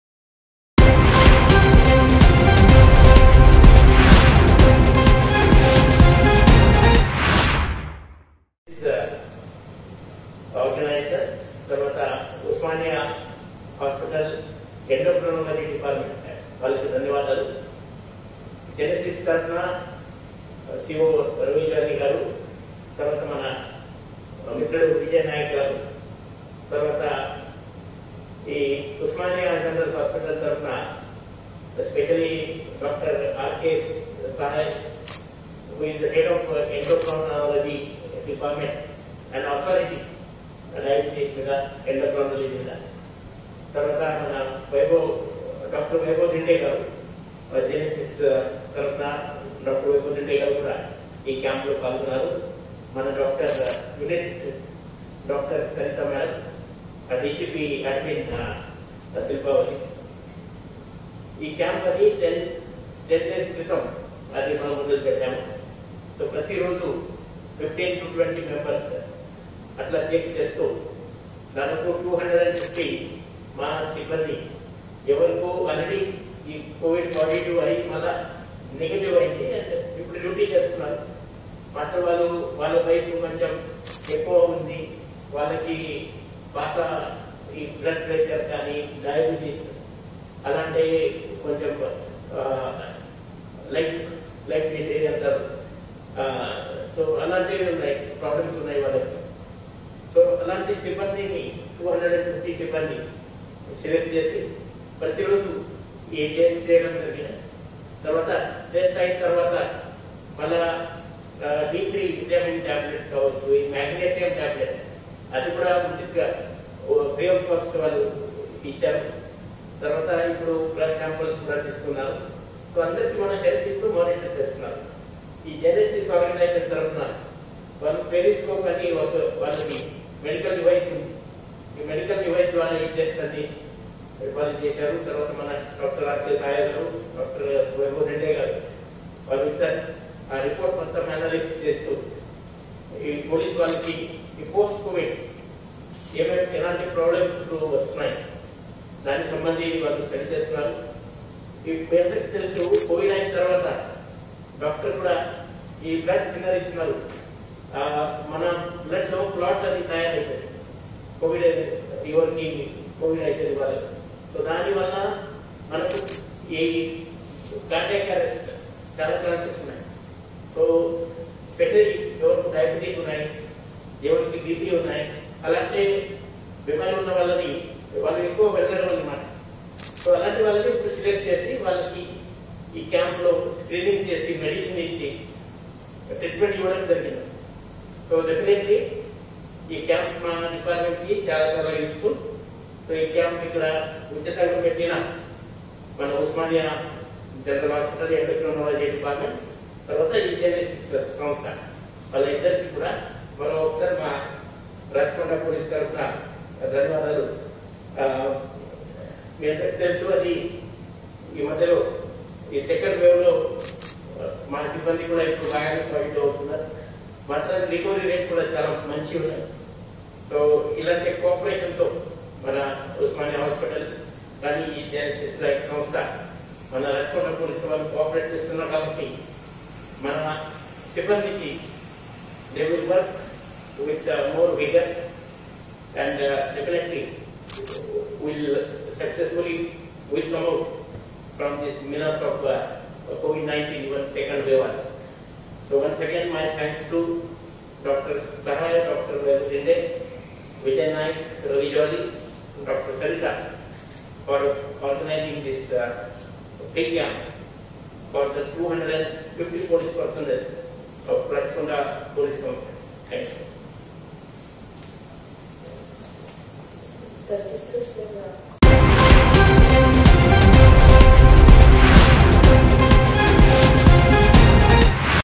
Osmania General Hospital conducts Rachakonda police health camp. || IndiaHeraldTV
Osmania General Hospital conducts Rachakonda police health camp.